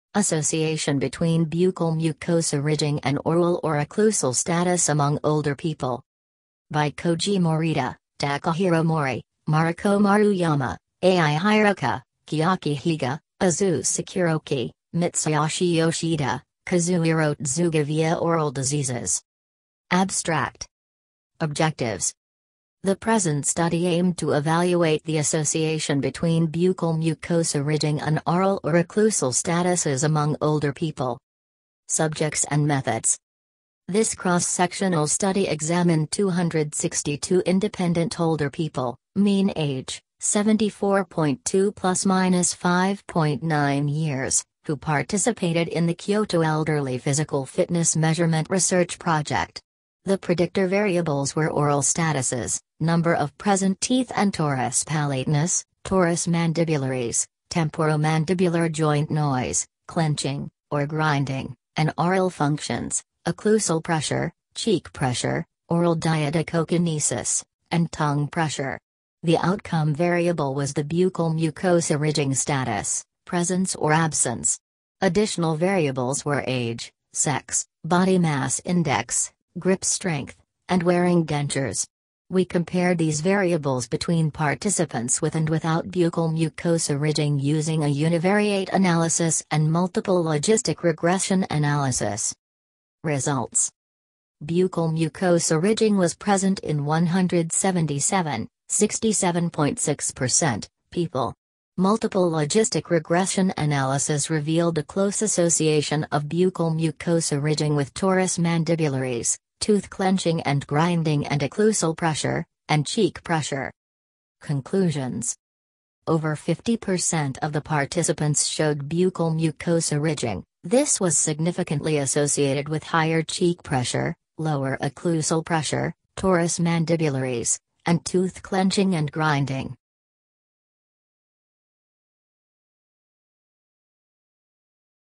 Buccal mucosa ridging and oral or occlusal statuses
Association between buccal mucosa ridging and oral or occlusal status among older people
by Koji Morita, Takahiro Mori, Mariko Maruyama, Aya Hiraoka, Chiaki Higa, Azusa Kuroki, Mitsuyoshi Yoshida, Kazuhiro Tsuga via Oral Diseases
Abstract
Objectives
The present study aimed to evaluate the association between buccal mucosa ridging and oral or occlusal statuses among older people.

Subjects and Methods
This cross-sectional study examined 262 independent older people (mean age, 74.2 ± 5.9 years) who participated in the Kyoto Elderly Physical Fitness Measurement Research Project. The predictor variables were oral statuses (number of present teeth and torus palatinus, torus mandibularis, temporomandibular joint noise, clenching, or grinding) and oral functions (occlusal pressure, cheek pressure, oral diadochokinesis, and tongue pressure). The outcome variable was the buccal mucosa ridging status (presence or absence). Additional variables were age, sex, body mass index, grip strength, and wearing dentures. We compared these variables between participants with and without buccal mucosa ridging using a univariate analysis and multiple logistic regression analysis.

Results
Buccal mucosa ridging was present in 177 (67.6%) people. Multiple logistic regression analysis revealed a close association of buccal mucosa ridging with torus mandibularis, tooth clenching and grinding and occlusal pressure, and cheek pressure.

Conclusions
Over 50% of the participants showed buccal mucosa ridging; this was significantly associated with higher cheek pressure, lower occlusal pressure, torus mandibularis, and tooth clenching and grinding.